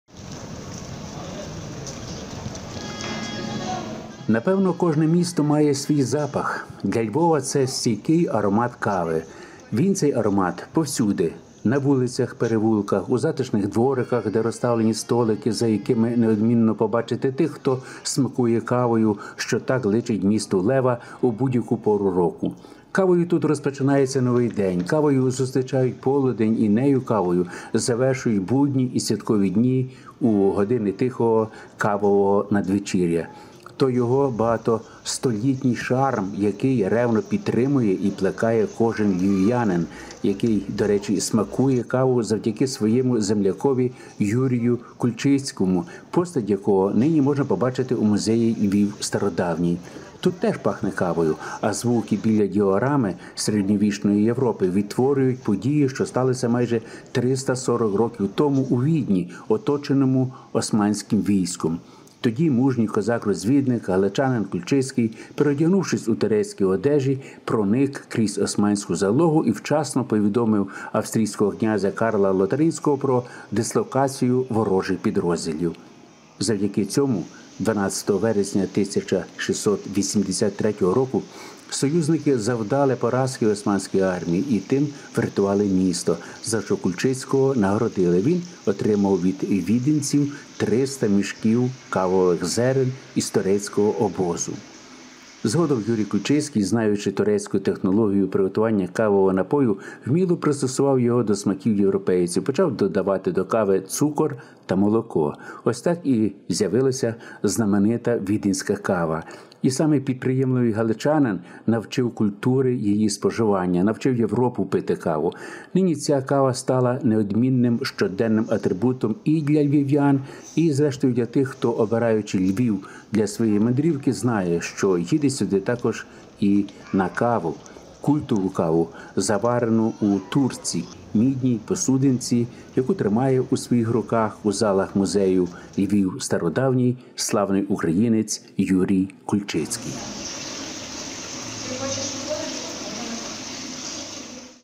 Історія Львівської кави
Напевно, кожне місто має свій запах. Для Львова – це стійкий аромат кави. Він, цей аромат – повсюди: на вулицях-перевулках, у затишних двориках, де розставлені столики, за якими – неодмінно побачите тих, хто смакує кавою, що так личить місту Лева у будь-яку пору року.

У такі самі вересневі дні, майже 340 років тому, перші кавові зерна привіз з Австрії до себе додому галичанин Юрій-Франц Кульчицький, який назавжди увійшов в історію, не тільки як видатний герой та рятівник Відня, але й людина, яка навчила Європу смакувати кавою. Про це, як свідчить репортаж нашого корепонрдента, пам’ятають і у Львові.